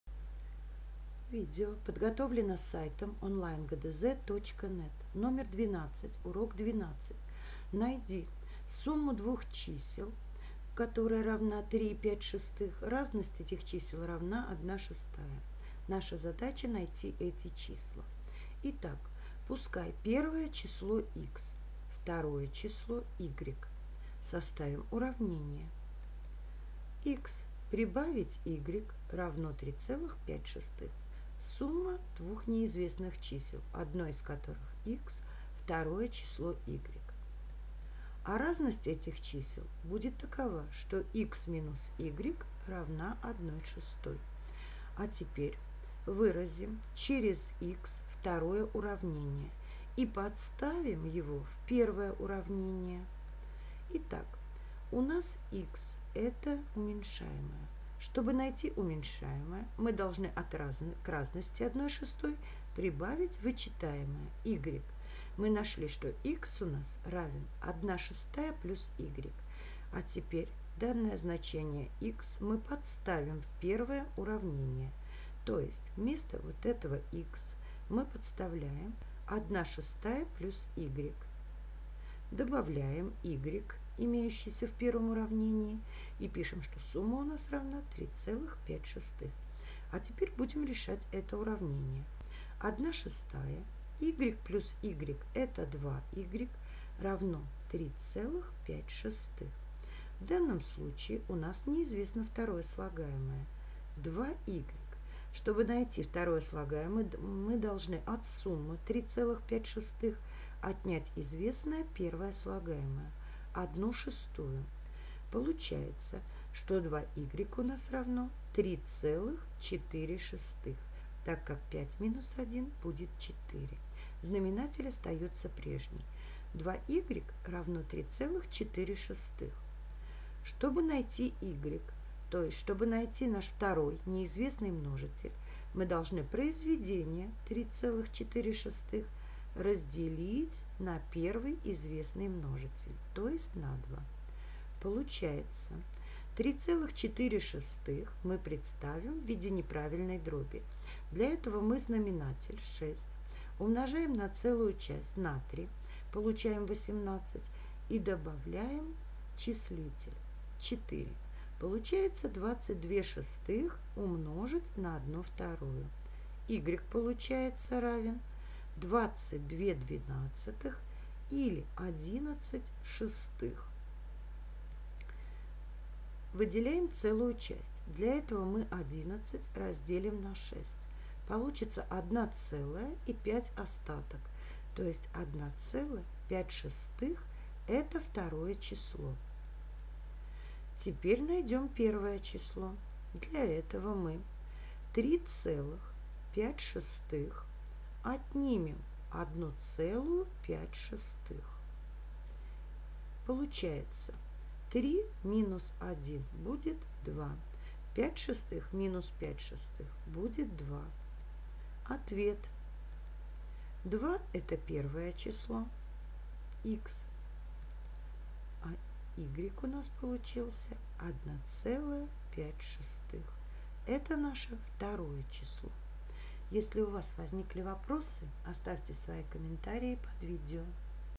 Урок 12 Задание 12 (б) – ГДЗ по математике 4 класс (Петерсон Л.Г.) Часть 3
В данном номере находим неизвестные числа по заданным условиям.